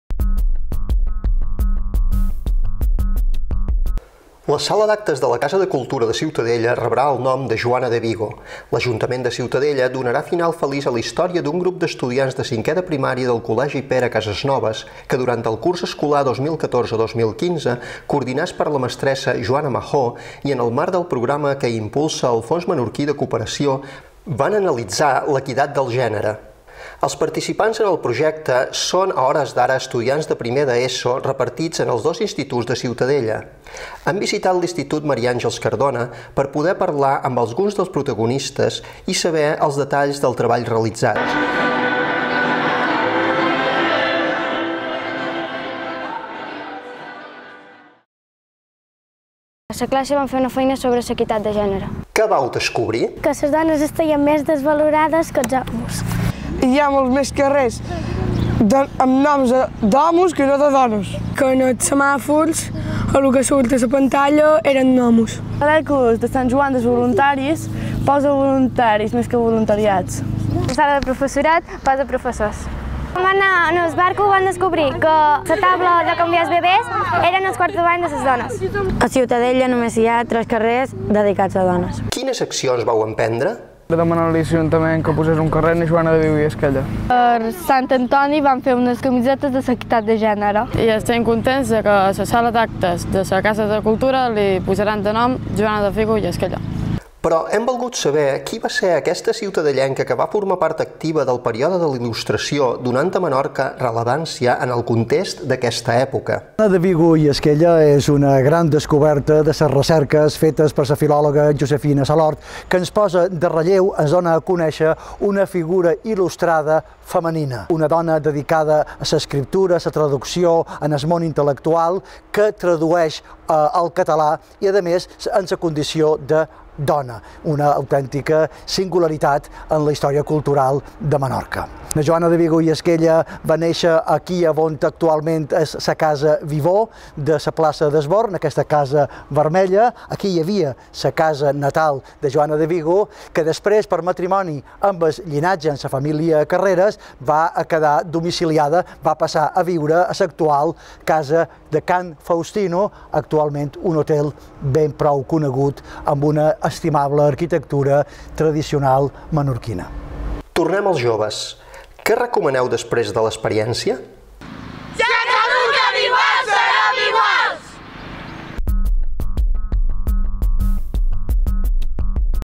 Joana de Vigo i Esquella, posarà el nom de la sala d'actes de la Casa de Cultua de Ciutad
La sala d'actes de la Casa de Cultura de Ciutadella rebrà el nom de Joana de Vigo

L'Ajuntament de Ciutadella, donarà un final feliç a la història d'un grup d'estudiants de cinquè de primària del Col•legi Pere Casesnovas, que durant el curs escolar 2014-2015; coordinats per la mestra Joana Majó i en el marc del programa que impulsa el Fons Menorquí de Cooperació; van analitzar l'equitat de gènere.
Els participants en el projecte, són a hores d'ara estudiants de primer d'ESO repartits entre els dos instituts de Ciutadella. Hem visitat l'Institut Mari Àngels Cardona, per poder parlar amb alguns dels protagonistes, y saber els detalls del treball realitzat.
Què van descobrir?
Quines accions van prendre?
Quins resultats han obtingut?
Però hem volgut saber, qui va ser aquesta ciutadellenca que va formar part activa en el període de la Il•lustració, donant a Menorca rellevància en el context d'aquesta època. Hem preguntat a Miquel Àngel Limón, President del Consell Científic de l'IME
Tornem als joves, Què recomaneu després de l'experiència?: “Si ens educan iguals, serem iguals”